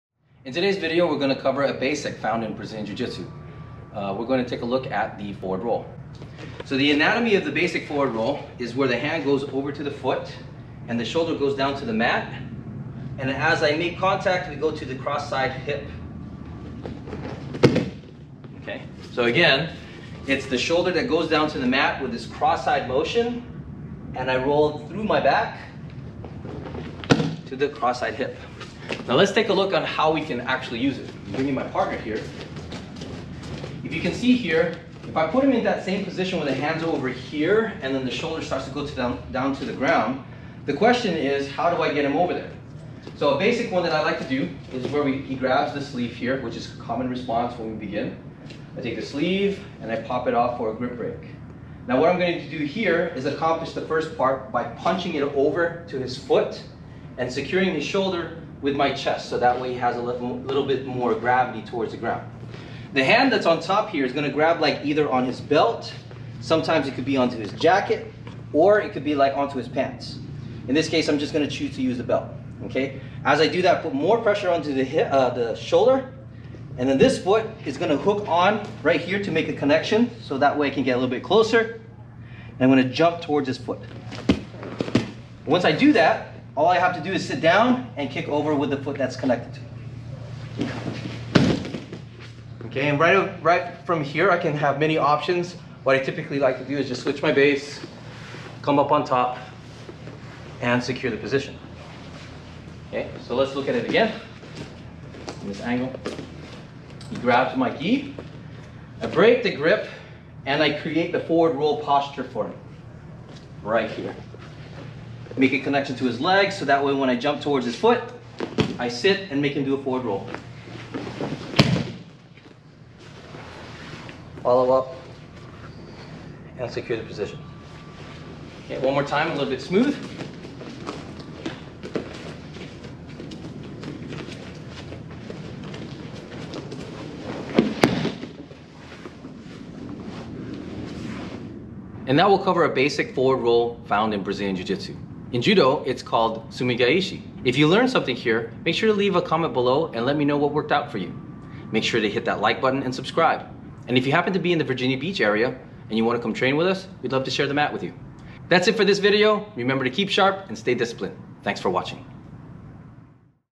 How to Use the Forward Roll in BJJ | Judo Throw Tsumi Gaeshi
In this video, you’ll find how to do a basic forward roll found in Brazilian Jiu Jitsu and how you can use it as a throwing technique.

Train with us in Virginia Beach at Impact Martial Arts Academy. Providing our community with top quality instruction for beginners and for veterans.